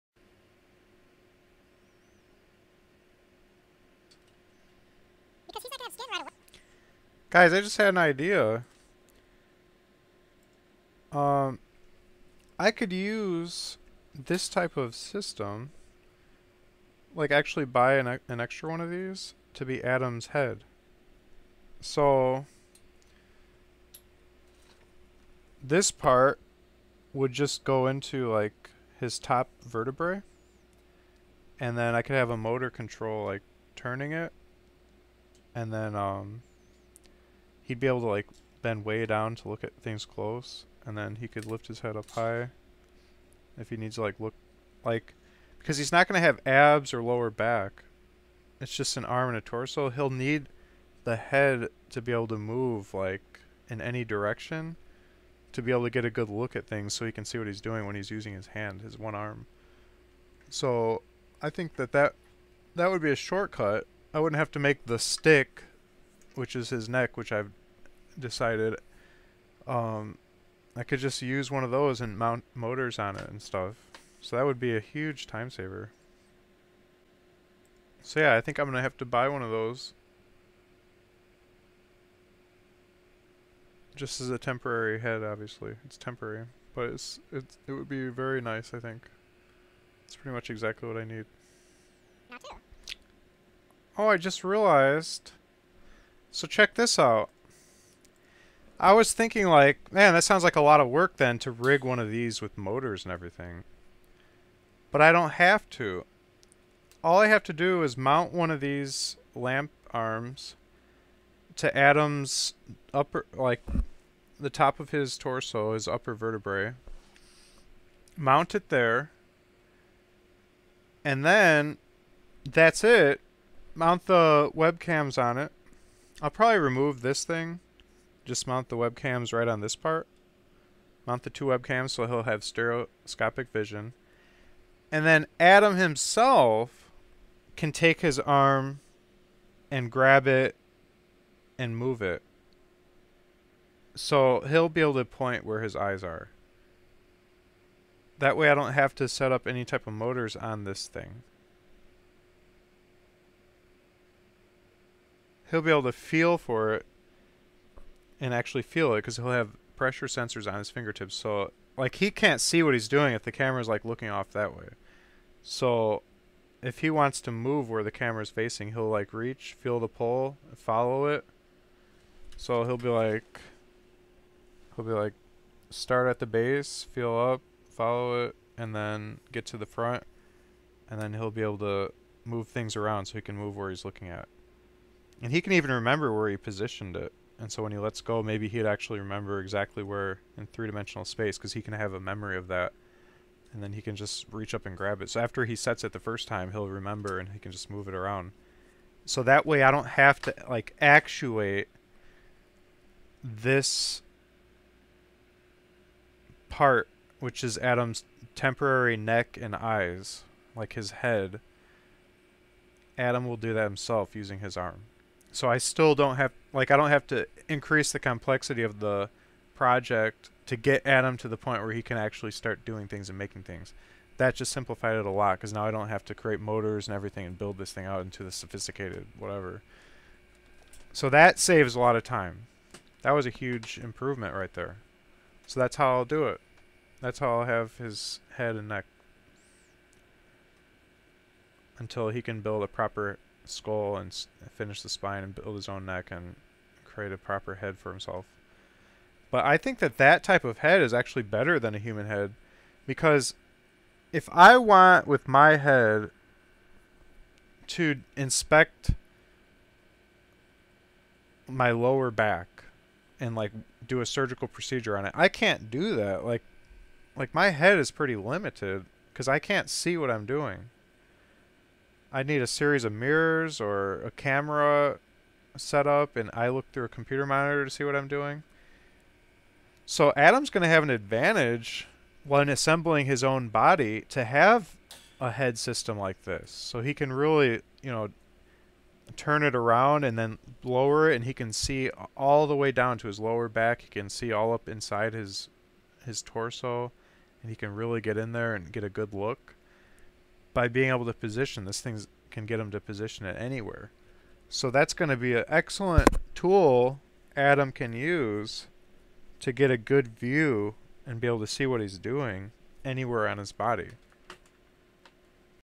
Plans to Use Desk Lamp as Stand in Head for My Robot | Building Humanoid Robots #131
In this video, I discuss my idea to use a desk lamp as a temporary placeholder head for my humanoid robot project. All the robot head really needs to do is position the robot's WebCam eyes onto whatever it is working on. Therefore, a boom arm desk lamp is ideal because of its large degree of freedom, axis of rotation, and adjustability. This will save me a lot of work and enable the robot see what it is doing even when it is working on its own back or midsection construction. Eventually, once the robot has finished building its own humanoid bone structure and body, a standard head and neck modeled after a human head and neck will be its head. But this desk lamp will serve as an excellent placeholder.